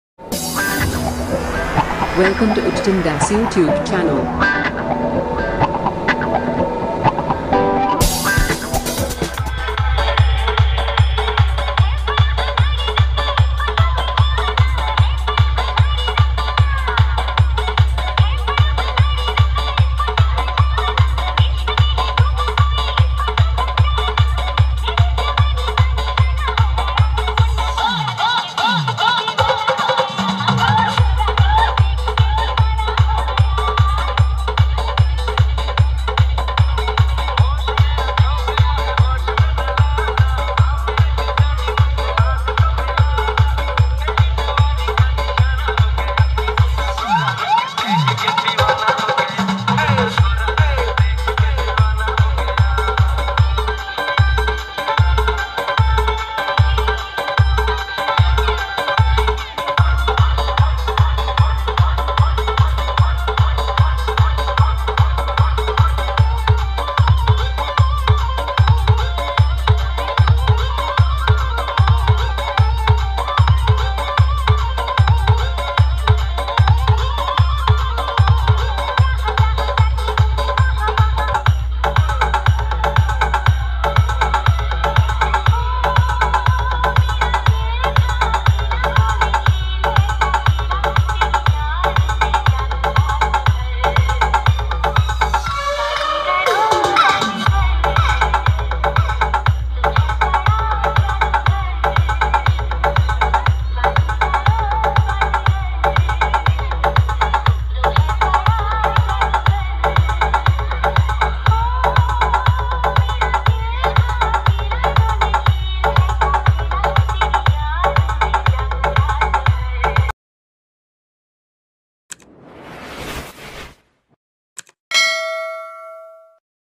সন্তোষী মিউজিক নারায়ণ পূজার উপলক্ষে হেঁড়িয়াতে চলছে
📀🎤এছাড়াও বিভিন্ন ধরনের গান ও Speaker Check এ ক্লাবের নাম ও Tag লাগানো হয় নিজেদের পছন্দের মতো Vocal দিয়ে 💿❤️❤️

Box competition...2022.